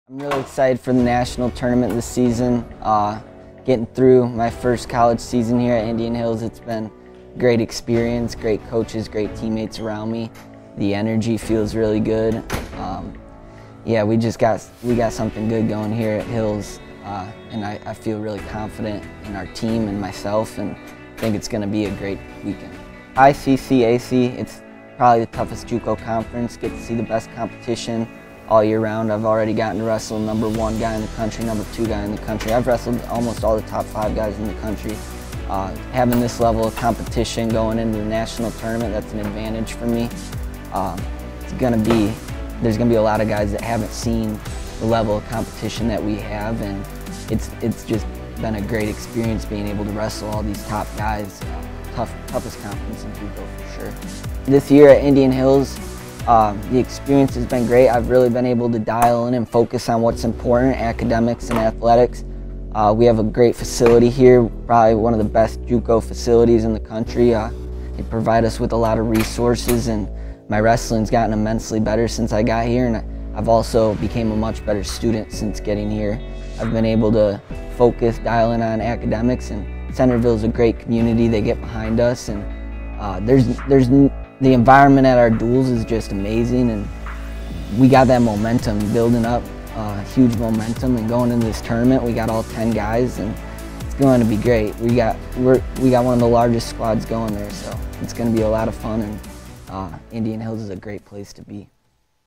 Indian Hills Men's Wrestling - Riley Bettich Previews 2023 NJCAA Nationals
Riley Bettich breaks down his first season at Indian Hills heading into the 2023 NJCAA Wrestling National Championship in Council Bluffs, IA.